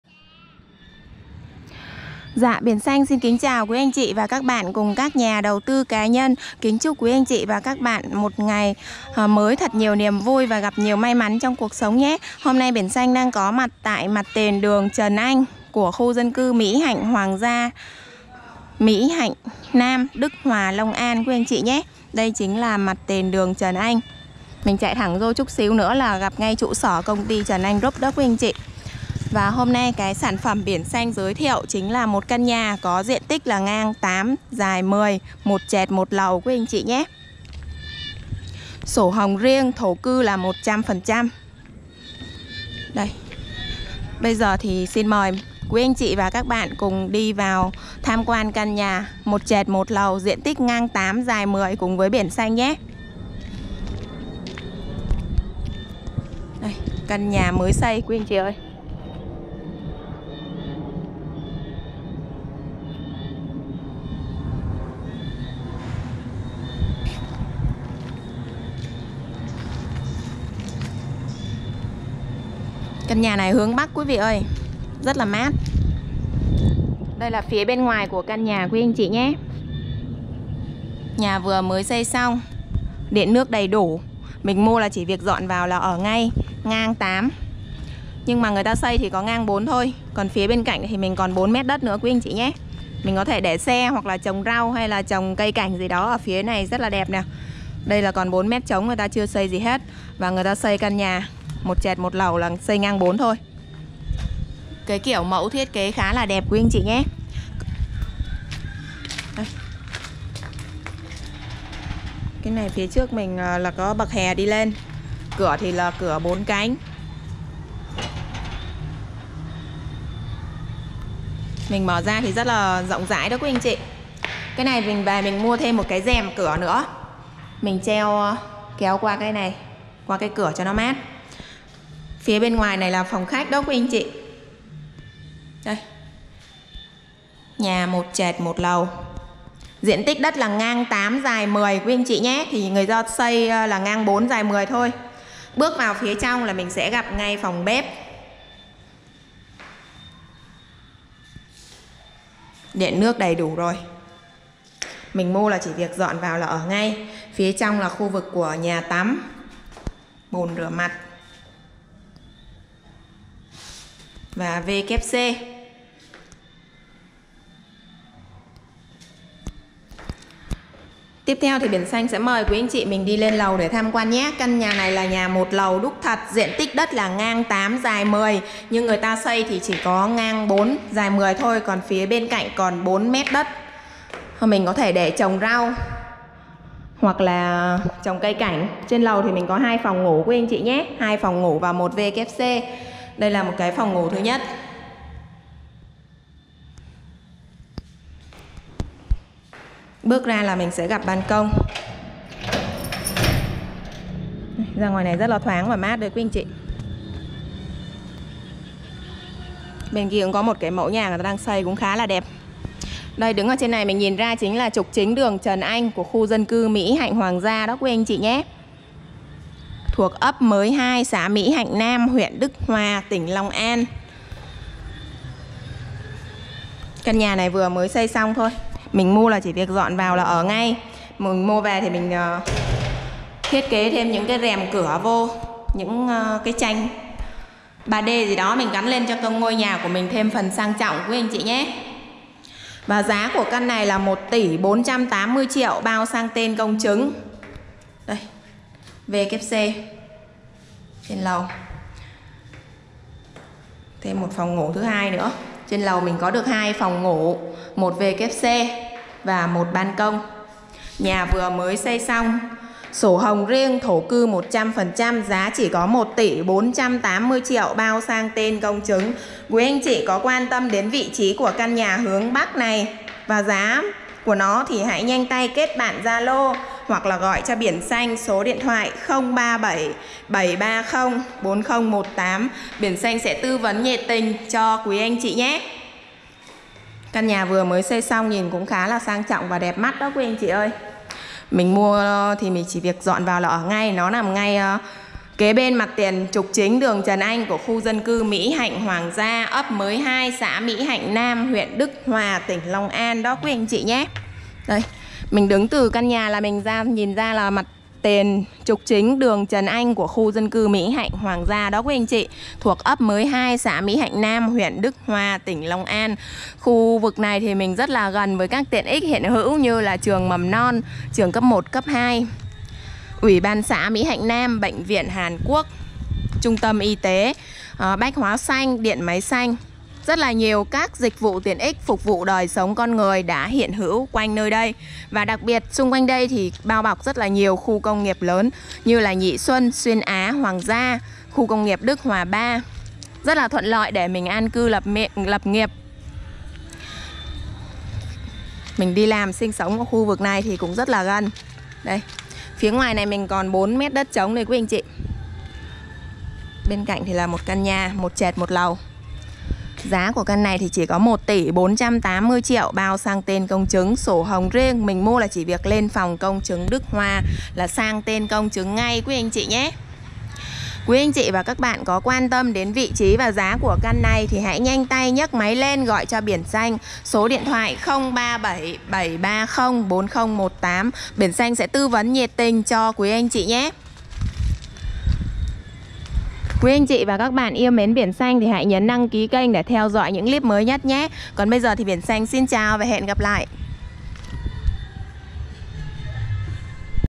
BÁN NHÀ LẦU MỸ HẠNH NAM- ĐỨC HÒA- LONG AN 01/07/2019
Cần bán căn nhà 1 lầu , diện tích đất 8x10,sổ hồng riêng, thổ cư 100%, nằm ngay kế bên mặt tiền trục chính đường Trần Anh, Chạy 150 mét là trụ sở Trần Anh group của KDC Mỹ Hạnh Hoàng Gia, Ấp Mới 2, Mỹ Hạnh Nam, Đức Hòa,Long An. Khu dân cư đông đúc, gần chợ, ủy ban xã, trường mầm non, cấp 1-2, bệnh viện hàn quốc, bách hóa xanh, điện máy xanh, Nhà Thờ Giáo Xứ Mỹ Hạnh, và bao bọc nhiều kcn lớn tạo công ăn việc làm , là nơi an cư lý tưởng. Giá 1 tỷ 480tr.